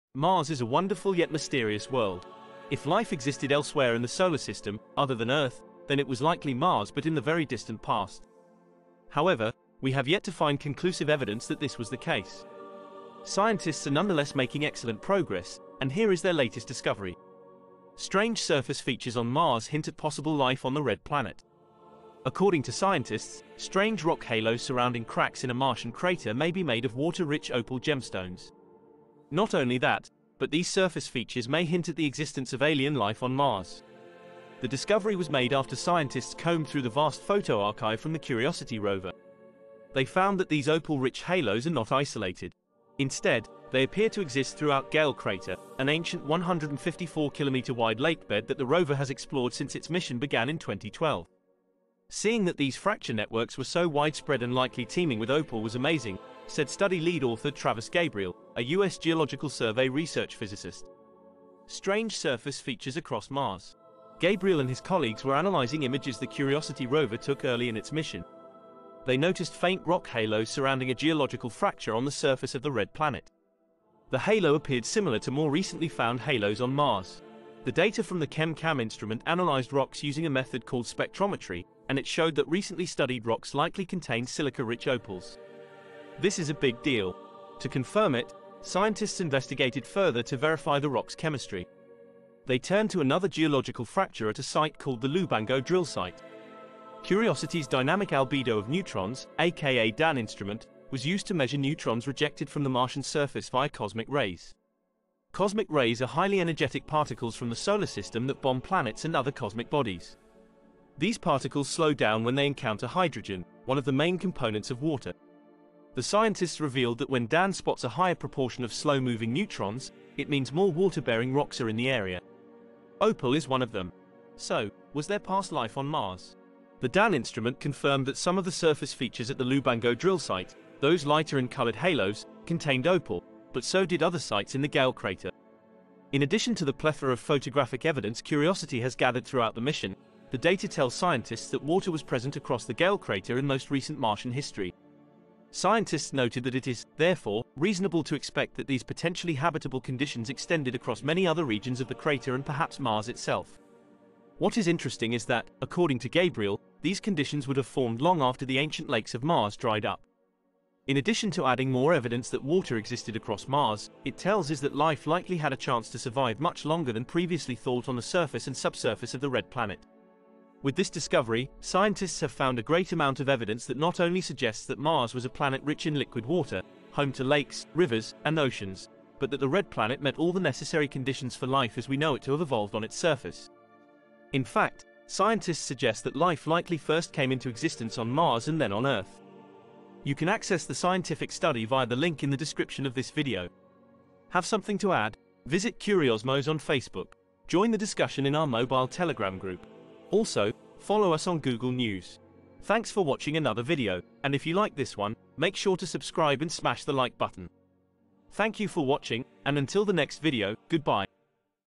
Strange Surface Features on Red Planet Hint at Past Life on Mars
Scientists investigating photographs taken by the Curiosity rover have identified strange surface features on Mars that hint at past life on the red planet.

Script: Written by Ivan Petricevic.